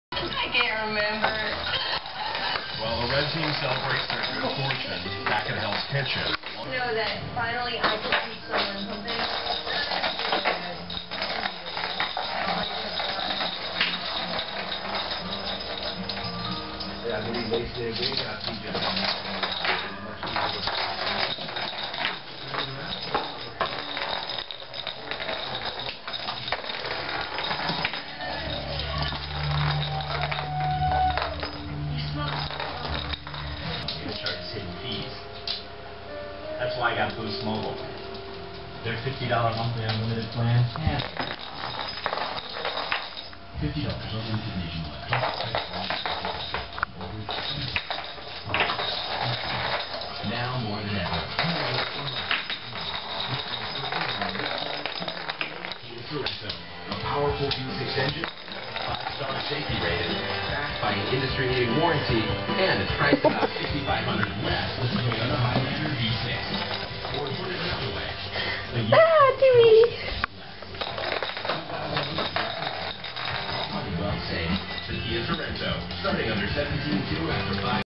Ferret playing ball ~ Tooo Cute~
Little Tubesock playing with her ball. She reminds me of a little seal when she does this :)